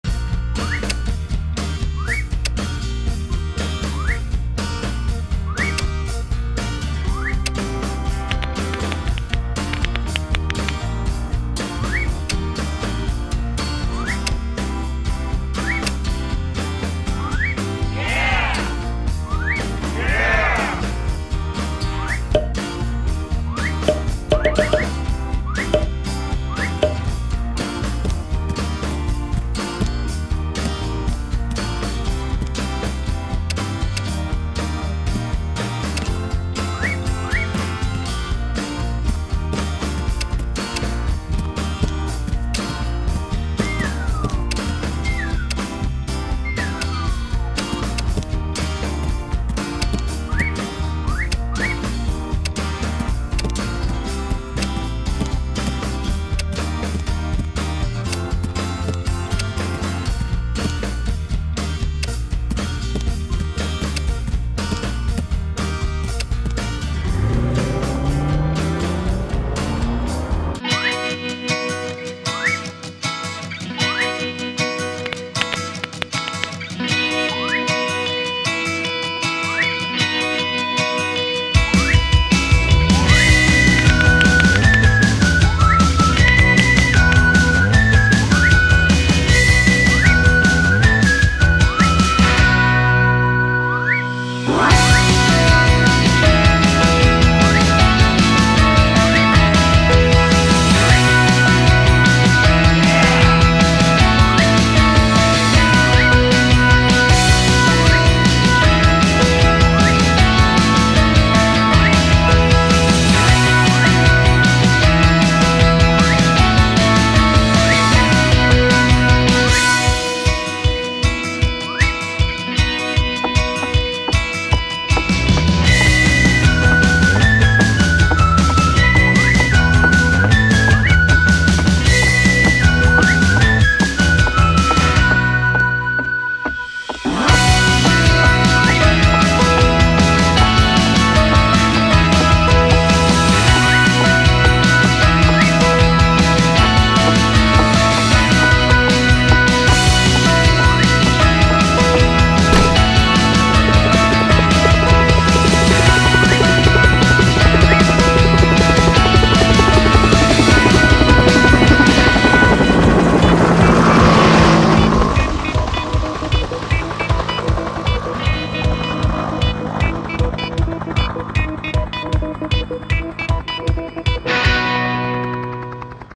Batman & Spiderman LEGO Brick Building Car Stop Motion Animation
Batman & Spiderman LEGO Brick Building Car with Superman & Ninjago & Star Wars Buld Helicopter Stop Motion Animation